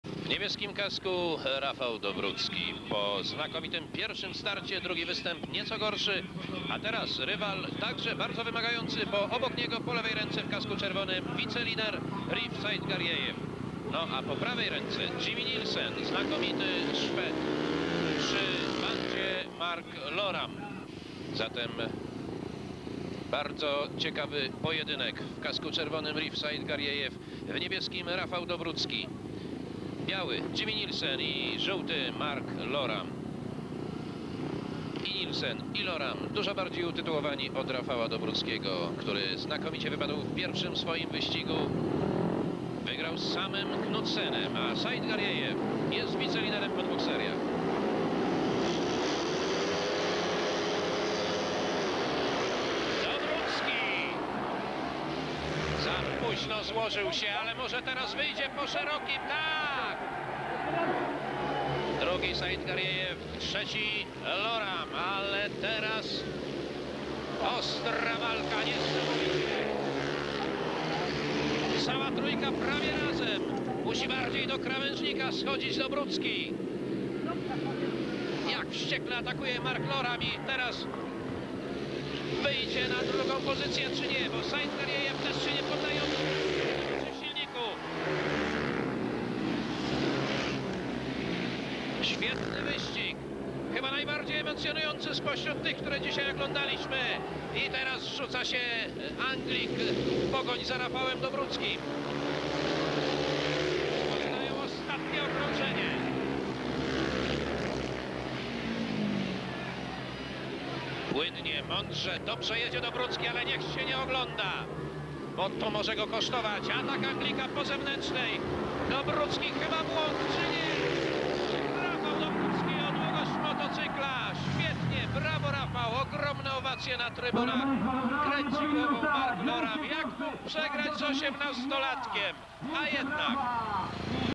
1994 Puchar Herosa - Warszawa Bieg 10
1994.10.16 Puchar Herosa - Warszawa
Bieg 10: Cz: Rif Saitgariejew, N: Rafał Dobrucki, B: Jimmy Nilsen, Ż: Mark Loram